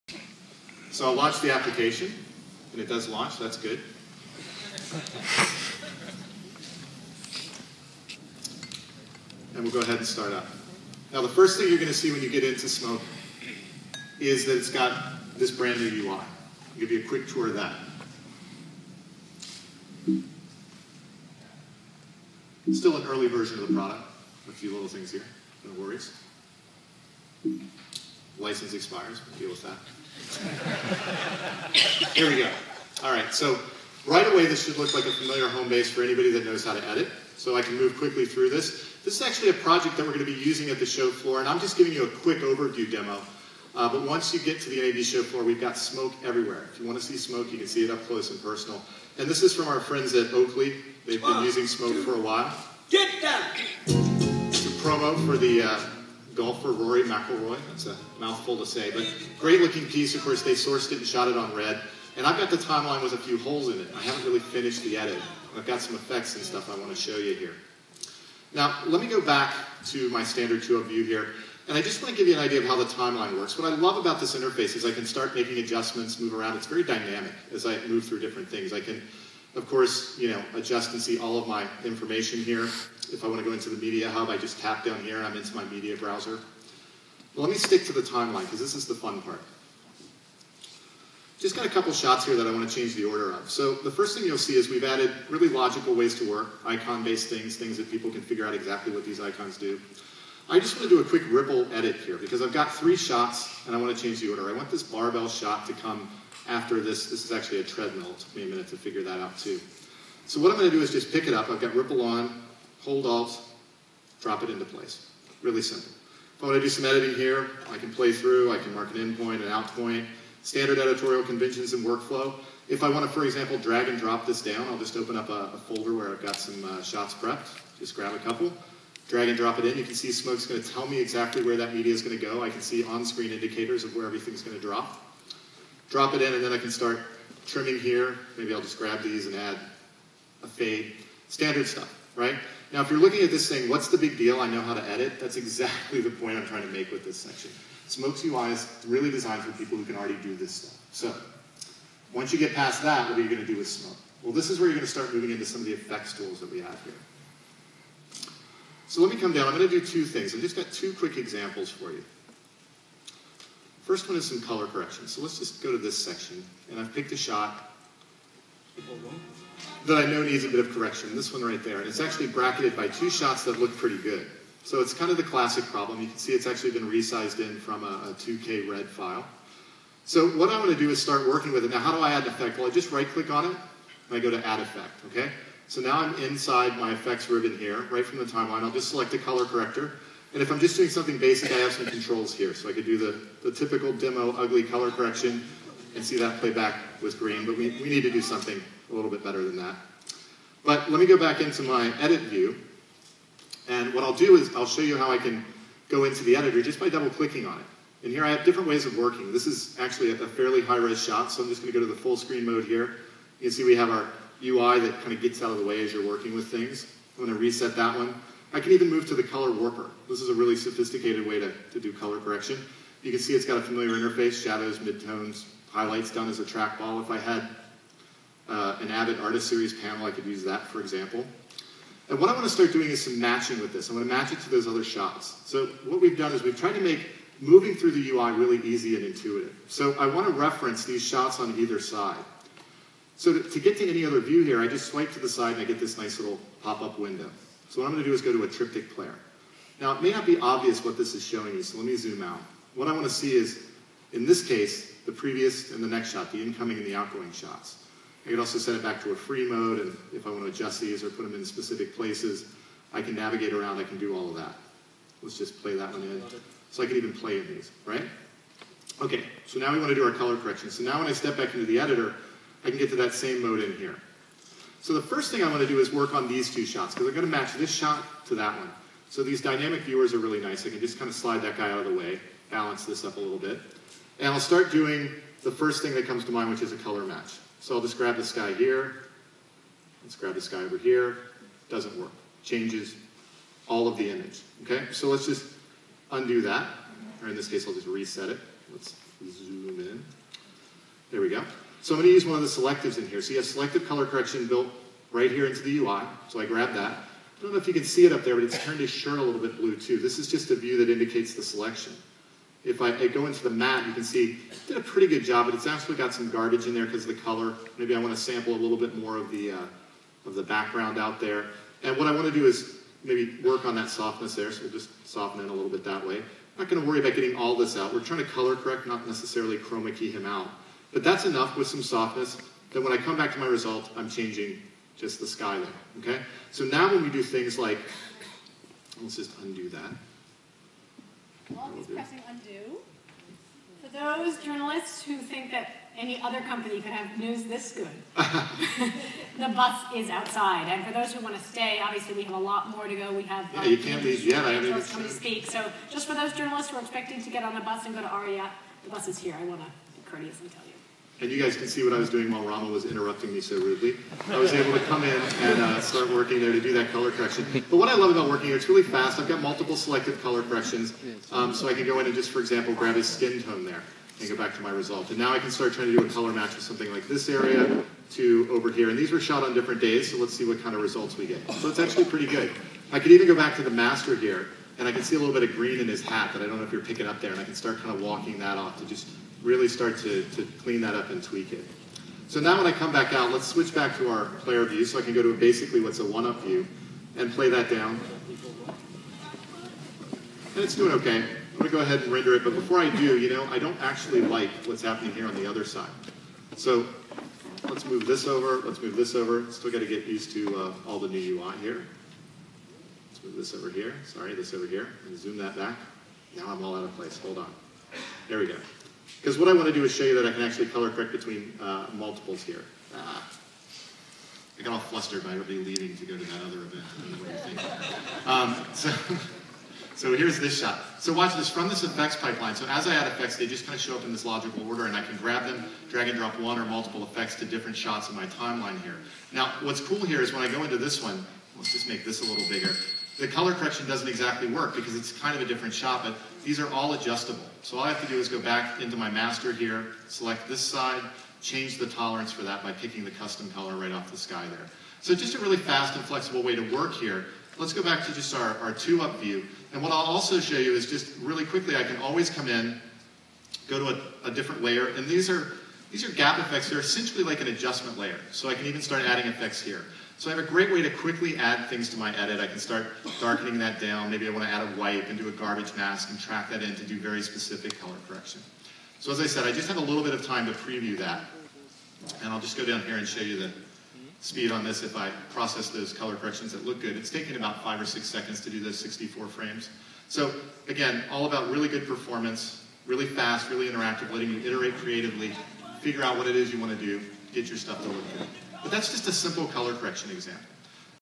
Marc Hamaker talking about color correction in the Smoke
Marc talks about basic color correction in Autodesk's new Smoke User Interface